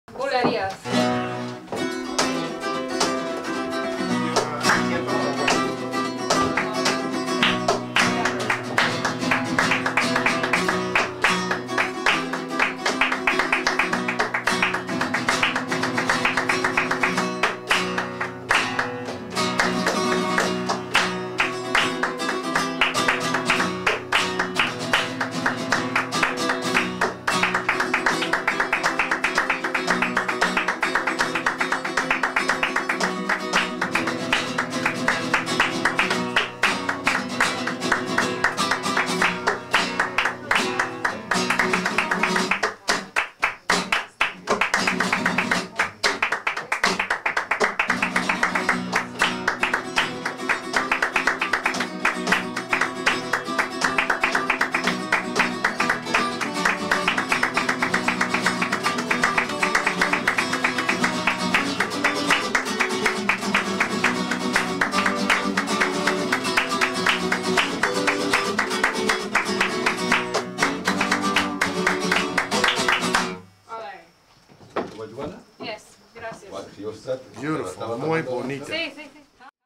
Bulerias Guitar with Palmas
Torcuato Zamora, who has played for the King  of Spain, plays some Bulerias for us in his home.  Miguel Perez, well known in the Flamenco community for his website dcflamenco.com, and his amazing Flamenco guitar playing, does the base bulerias palmas rhythm, clapping all 12 beats, with accents on 3,6,8,10 and 12,  while I play in between those beats (on the and), aka contra- rhythms.

I started to show where the base rhythms are, but it was too tedious when my ADD kicked in.  Hopefully you'll get the gist with my starting it off for you in the video- u cholos.

While Miguel keeps the base rhythm, with accents on beats 3, 6, 8, 10 and 12,  I do contra-rhythms, and sometimes do accents on different beats like 12,3,7,8,and 10, just to change it up a bit.  It's even more fun with more people.

Also note that the guitarist, Torcuato, often taps the guitar with his huge finger nails on certain beats, with stronger strumming mainly 3,6,8,10 and 12, but sometimes on other beats to change it up for added flair.
Notice that Flamenco numbers like the bulerias end on beat 10.  That's what makes the end of a Flamenco number so dramatico and un-expected.  More on how to tell when the end of a piece is coming later, and how to know when to end on beat 10.

They don't call me Miss Contra- tiempo for nothing.  lol.